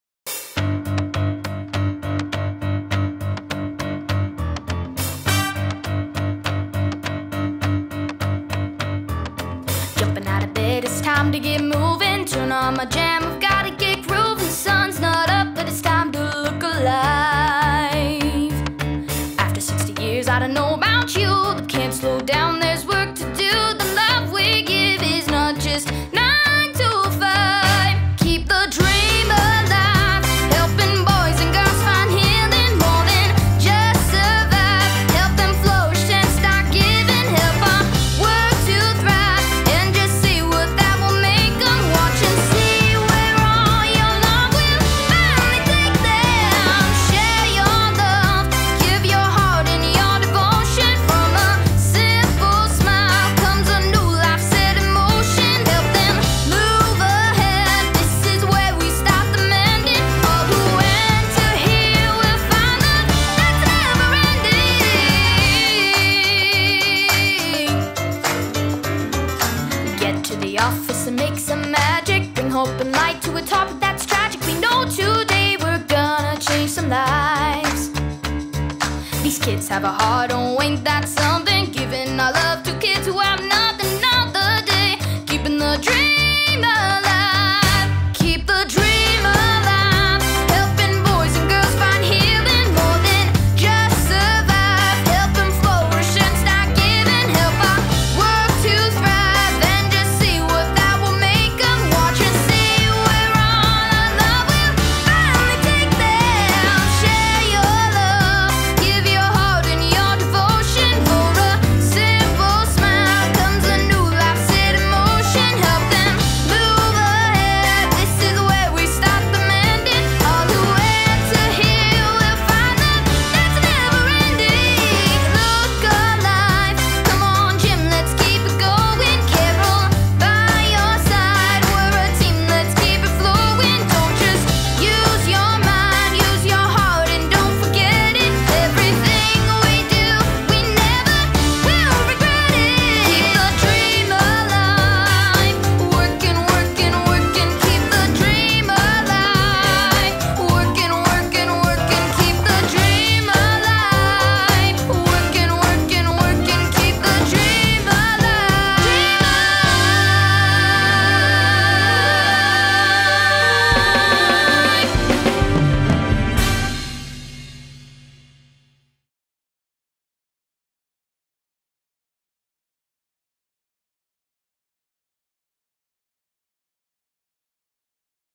9 to 5
Over the last 60 years, Childhelp Founders Sara O’Meara and Yvonne Fedderson have been working around the clock to fight child abuse and neglect. Childhelp Youth Ambassador Rosevelt Rawls shows us that their work is far from just “9 to 5".